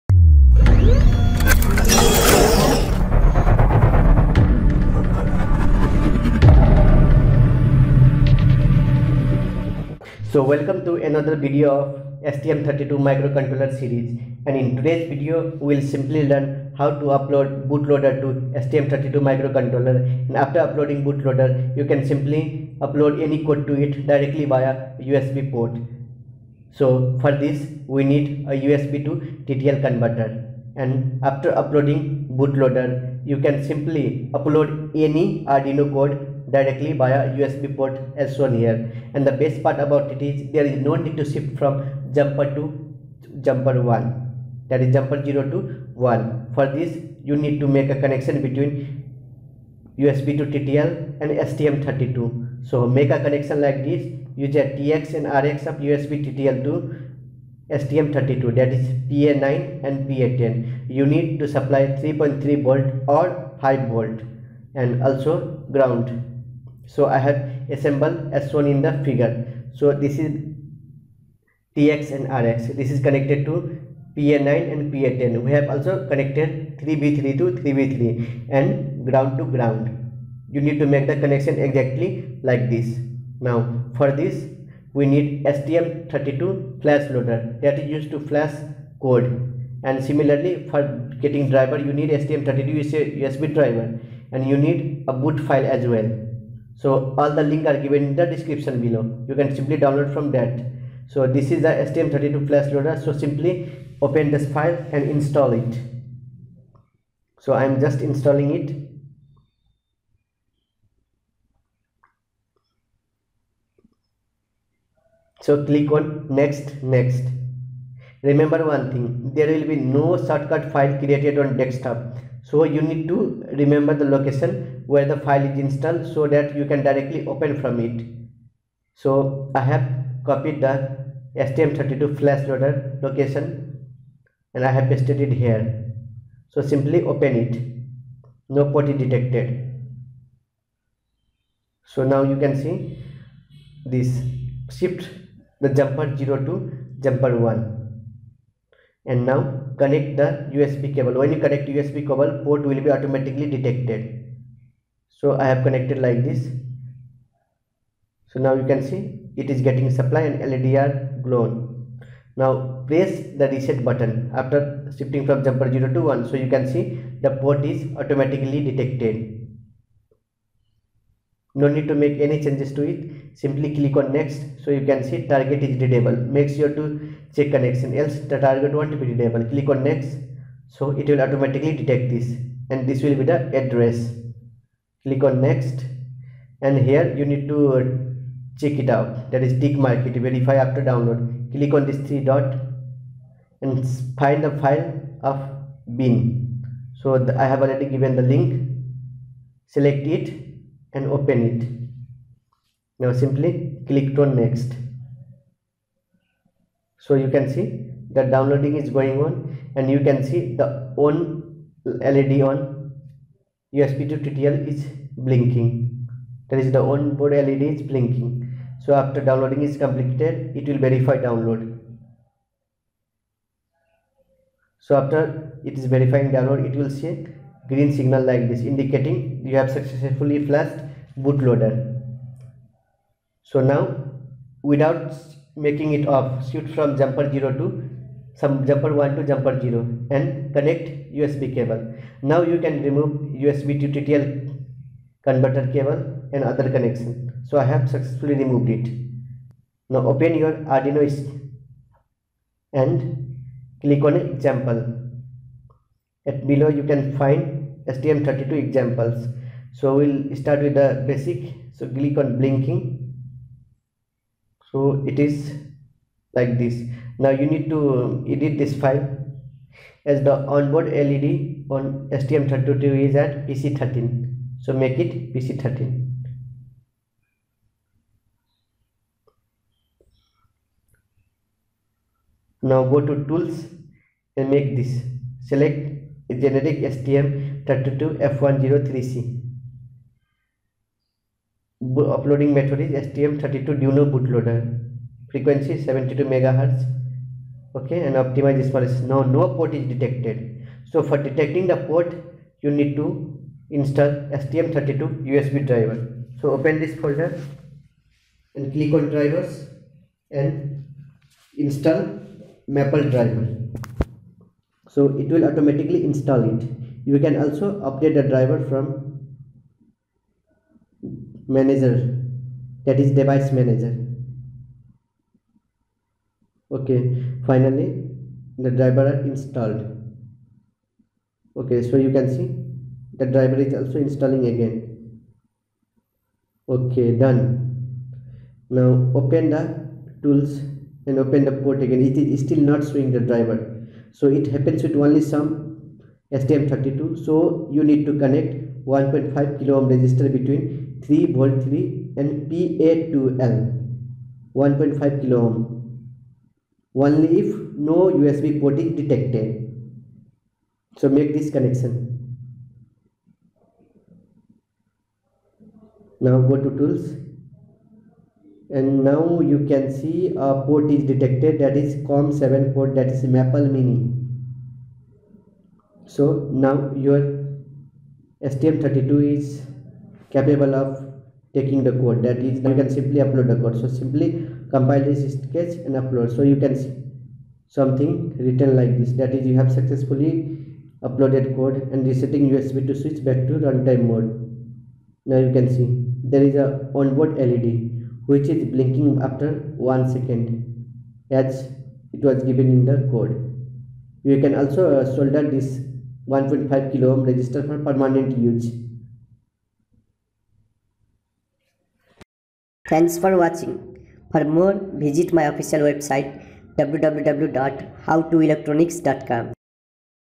STM32 Bootloader | Programming STM32F103C via USB Port
This video is all about installing STM32 Bootloader, i.e Programming STM32F103C8 Board using USB Port directly. STM32 Development Board or STM32F103C8 Microcontroller can be easily programmed using the Arduino IDE..

In our last tutorial we learnt the basics of the STM32 Development Board and also programmed it to blink an LED. But there was one huge drawback, i.e In order to program the Board we need a FTDI programmer module and also we have to toggle the boot 0 jumper between and 1 position while uploading and testing a code. Also the mini-USB port on the Development board was left totally unused. The reason for doing that is, when the STM32 development board is purchased it does not come with an Arduino ready boot loader and hence the board will not be discovered by your computer when connected through the USB.

But there exists a boot loader program developed by LeafLabs for Maple mini boards. This boot loader can be flashed into the STM32 once and thereafter we can directly use the USB port of the STM32 board to upload programs just like any other Arduino boards.

Link for STM32 Flash Loader, USB Driver and bootloader bin file